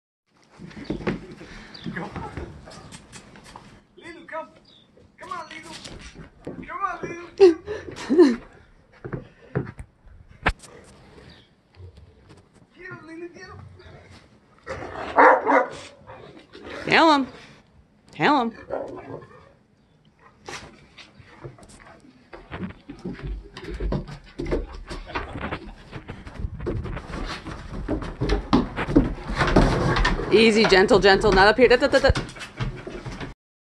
Great Danes playing in the snow
Great Danes, Leeloo and Cruz, playing in the snow. Their first real snow fall experience.